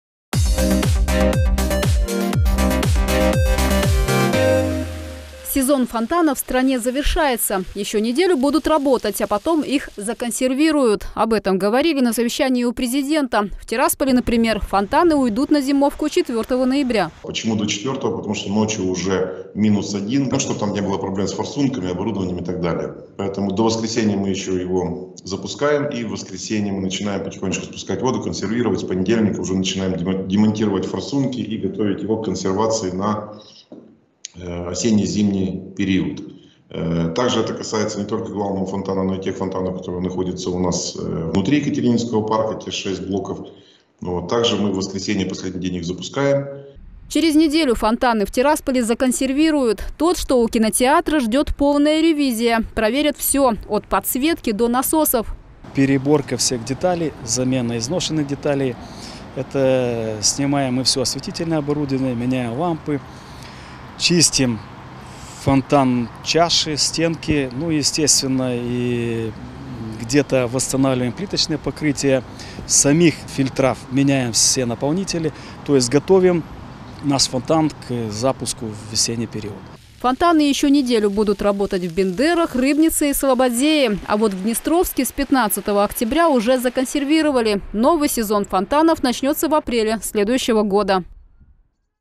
Сезон фонтанов завершается: через неделю – на зимовку
Сезон фонтанов в стране завершается. Еще неделю будут работать. А потом их законсервируют. Об этом говорили на совещании у Президента. В Тирасполе, например, фонтаны уйдут на зимовку 4 ноября.